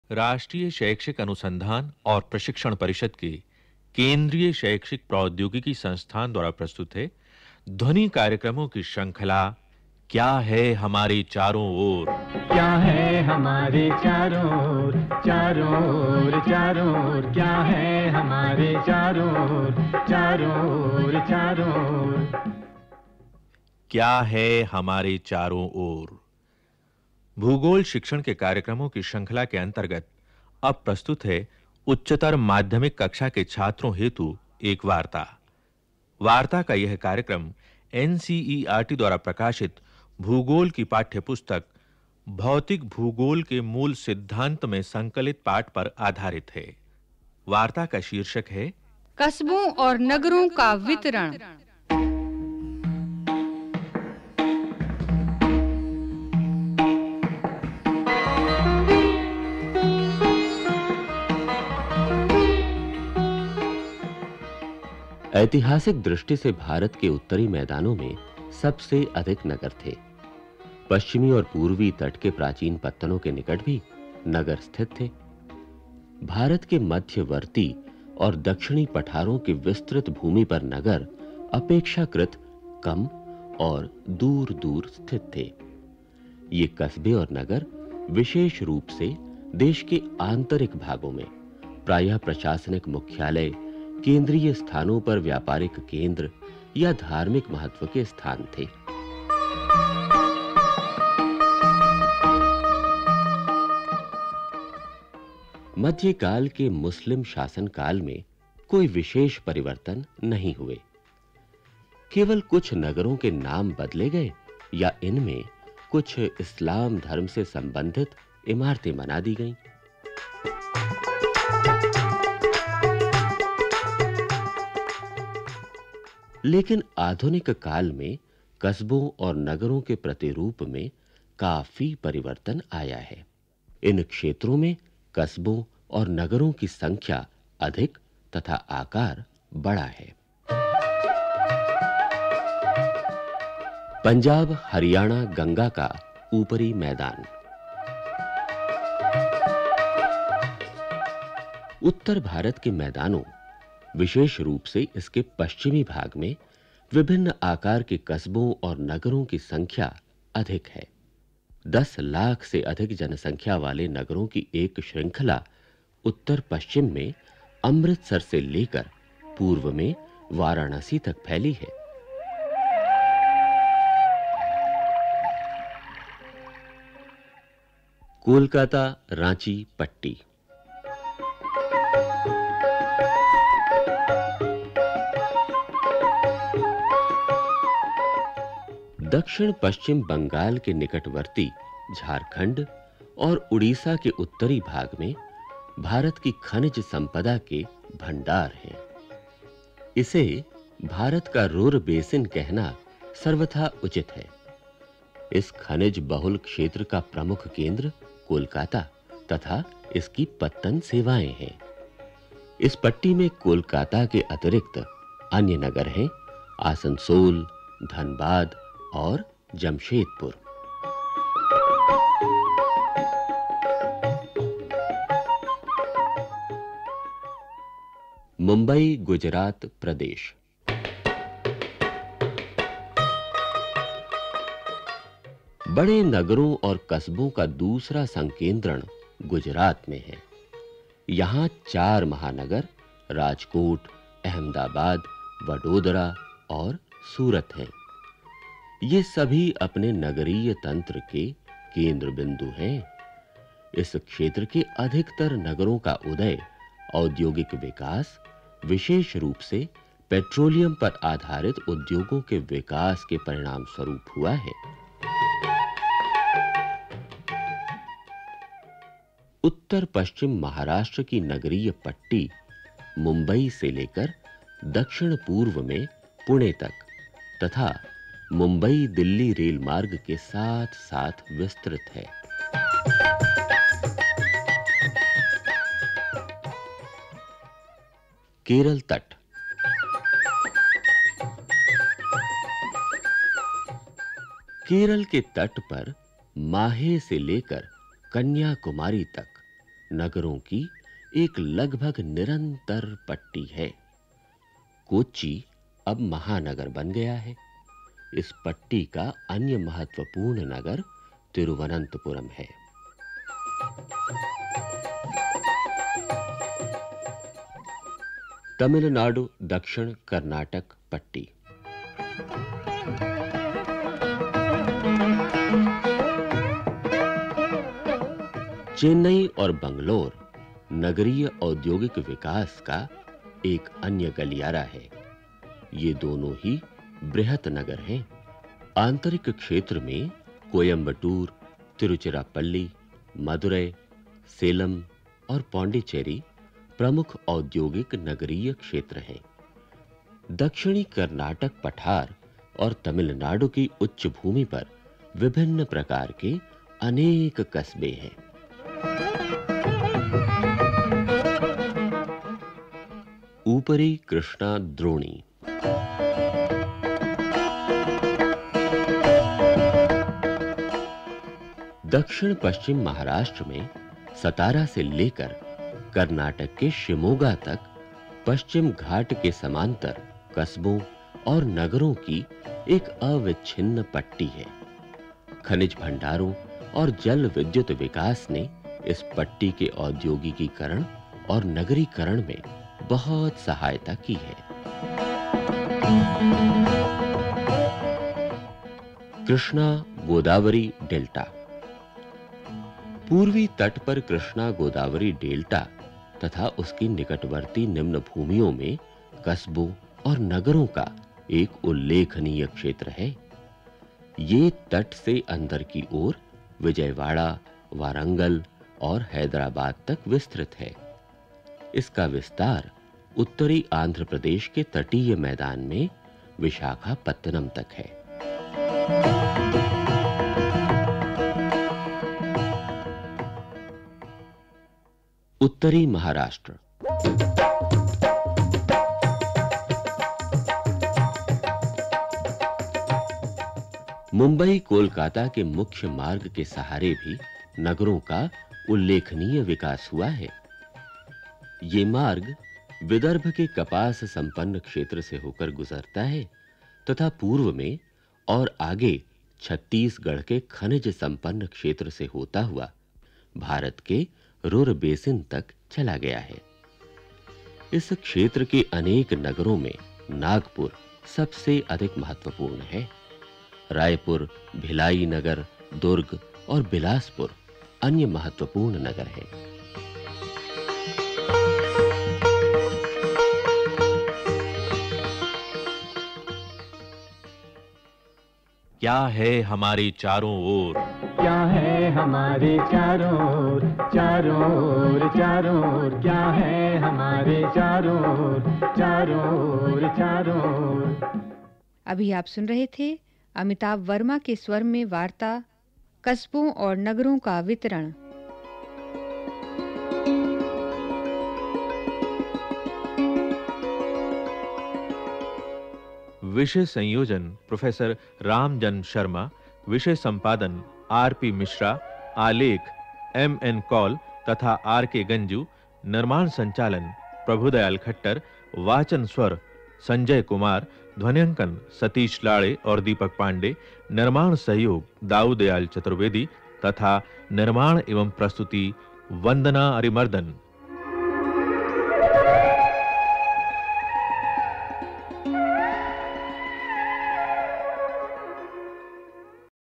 Kasbon Aur Nagron Ka Vitran /कस्बों और नगरों का वितरण
यह कार्यक्रम मूलतः एक भूगोल वार्ता है जिसके द्वारा भूगोल का विषय विस्तार समझाया गया है तथा उससे संबंधित अनेकानेक विषयों पर चर्चा की गयी है।